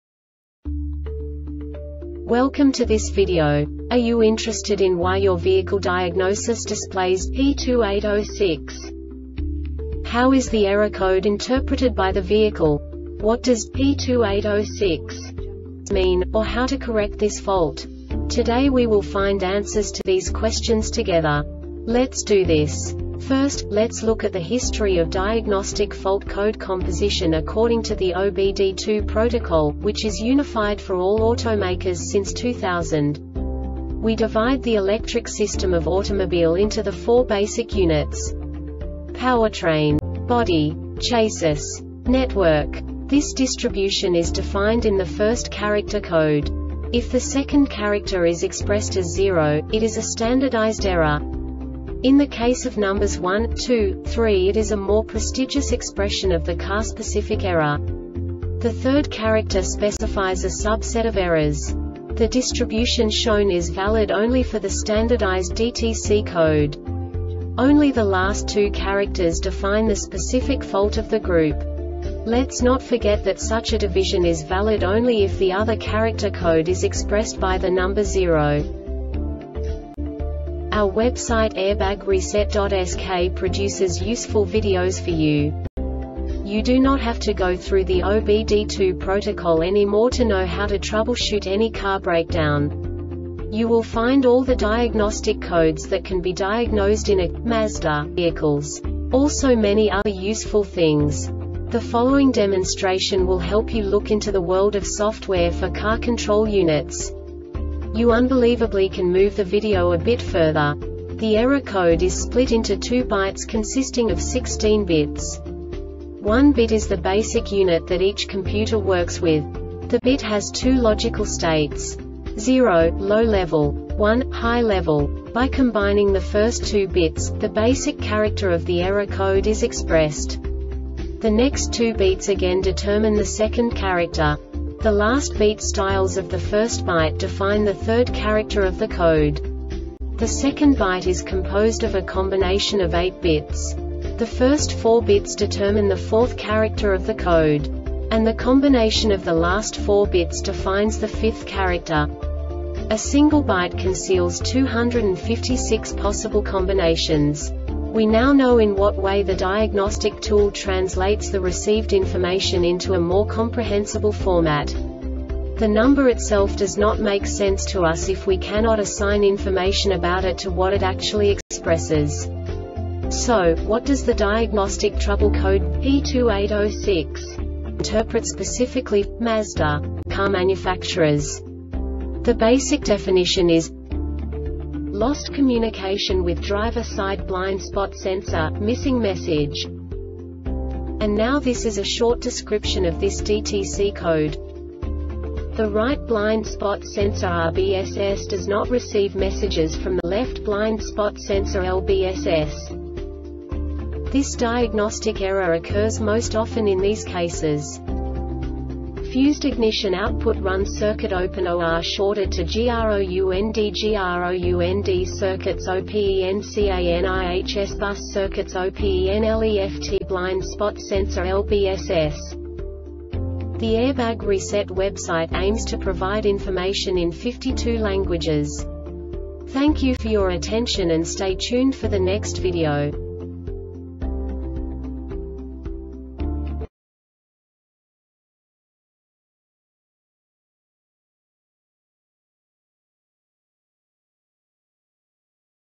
DTC Mazda P2806 Short Explanation
Contents:
0:21 Basic DTC analysis according to OBD2 protocol standard.
1:48 Insight into programming
2:58 A diagnostically understandable description of the Diagnostic Trouble Code
3:05 Basic error definition

Make:
Mazda
Code:
P2806
Definition:
Lost Communication With Driver Side Blind Spot Sensor - Missing Message
Description:
The Right Blind Spot Sensor (RBSS) does not receive messages from the Left Blind Spot Sensor (LBSS).
Cause:
FUSED IGNITION OUTPUT (RUN) CIRCUIT OPEN OR SHORTED TO GROUNDGROUND CIRCUIT(S) OPENCAN IHS BUS CIRCUIT(S) OPENLEFT BLIND SPOT SENSOR (LBSS)